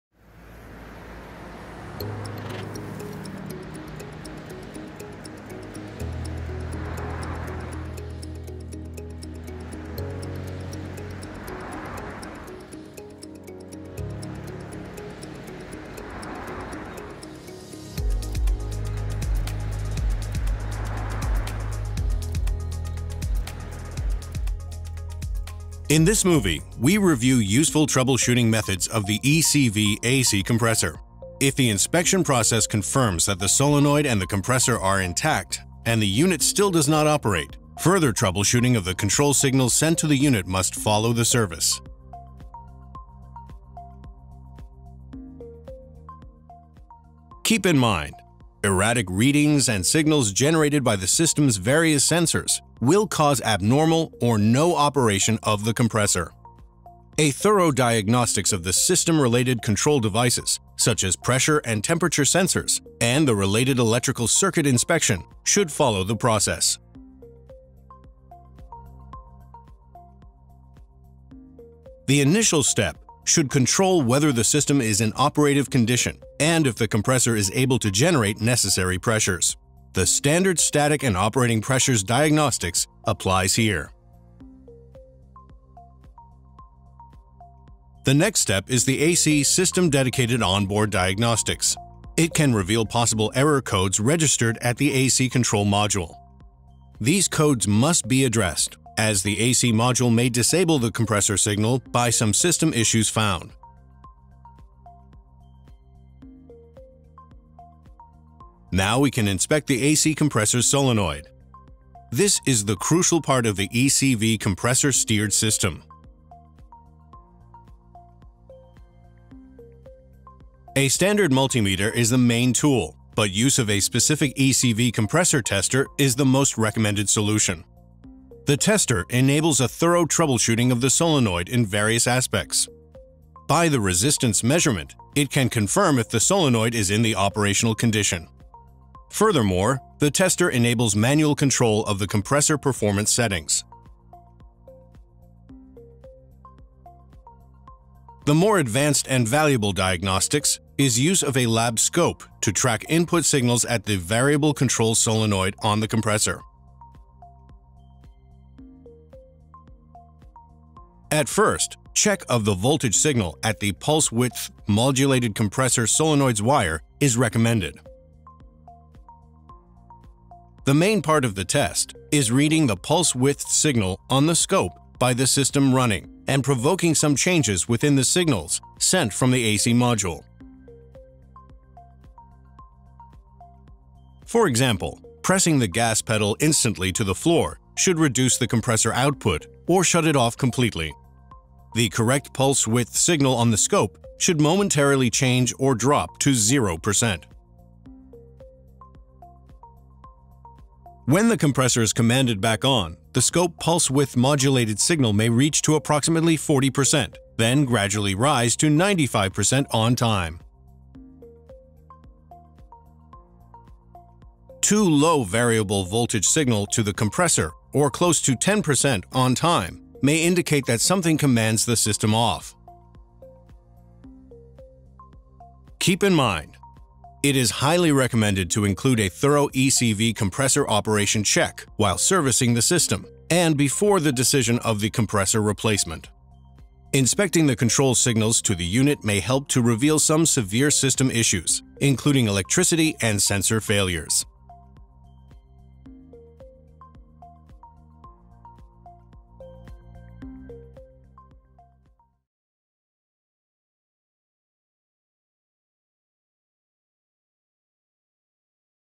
ECV AC Compressor - Solenoid and the Control Signals Troubleshooting
Best Service Practice Sharing - ECV AC Compressor

Solenoid and the Control Signals Troubleshooting

Testing performance of the compressor as well as inspection of the variable control solenoid and monitoring control signals can help to identify and fix some serious system failures that do not have root causes in compressor failures.

In this movie, we review useful troubleshooting methods of the ECV AC Compressor.